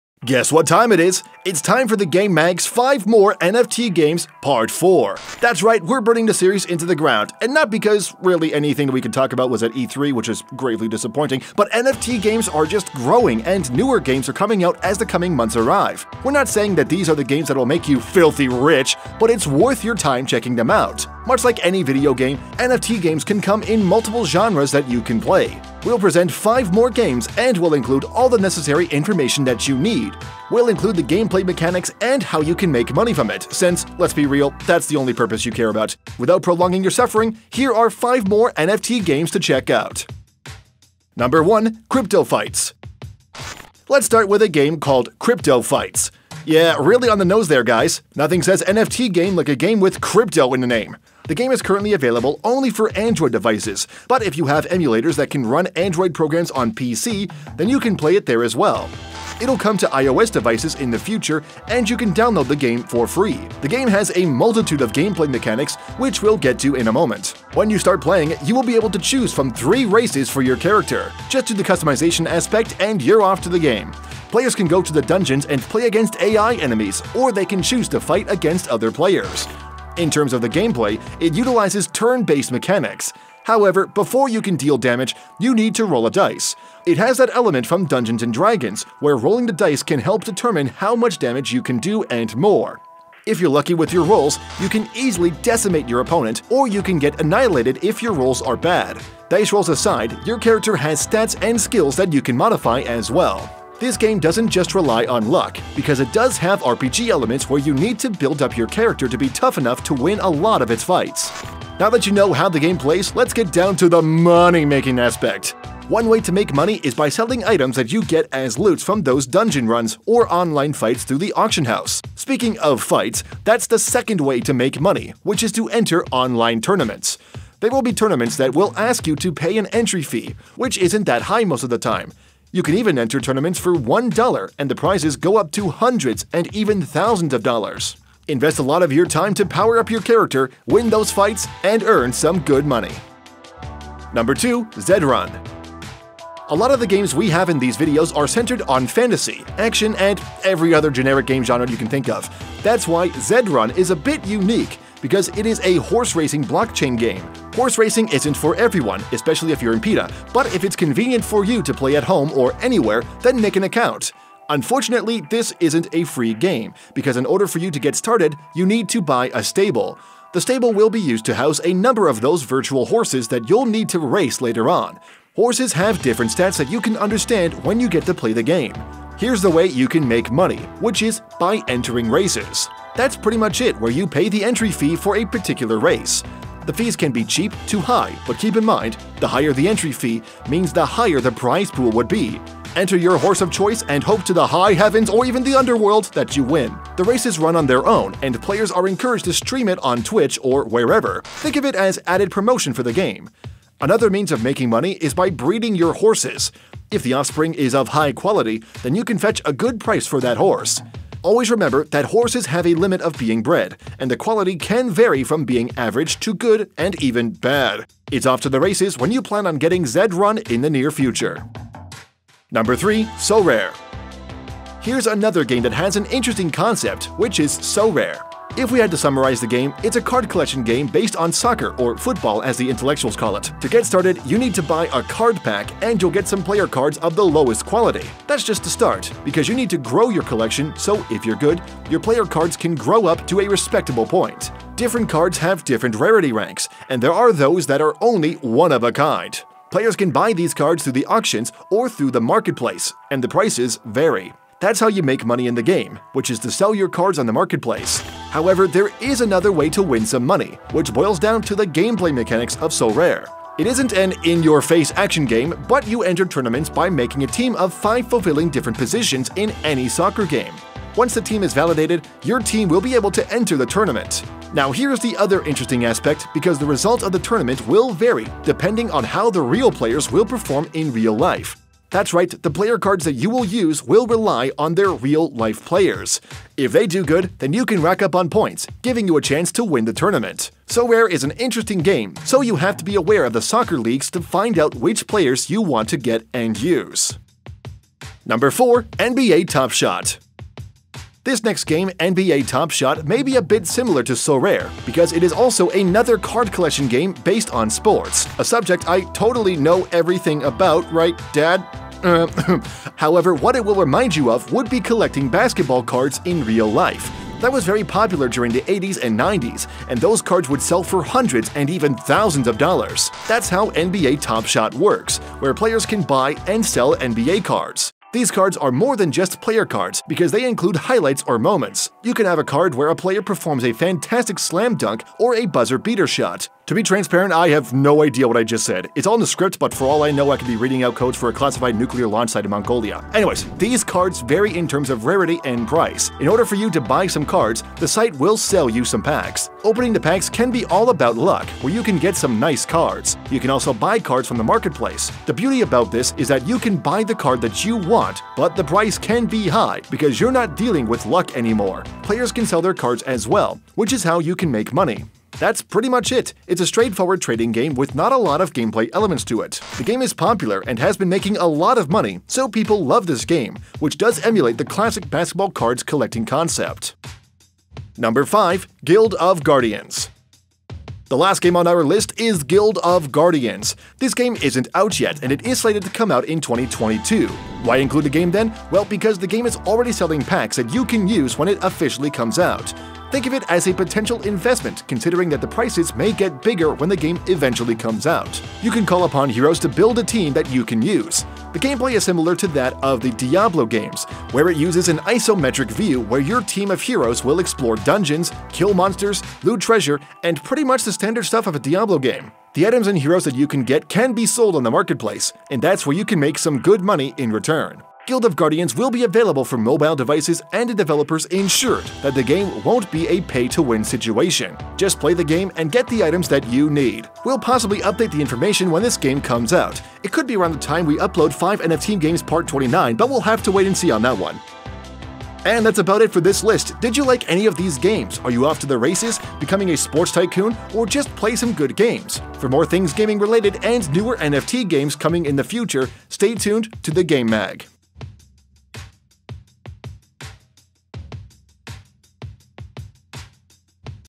Top 5 Crypto NFT Games | Play to Earn $ Crypto Blockchain Games
NFT games are just growing and newer games are coming out as the coming months arrive.
An NFT means non-fungible tokens and blockchains are recordings of your transactions online related to cryptocurrency and more. NFT on the other can be physical or digital assets that you can use to make money.
Much like any videogame, NFT games can come in multiple genres that you can play. We’ll present five more games and we’ll include all the necessary information that you need.
We’ll include the gameplay mechanics and how you can make money from it since that is the purpose.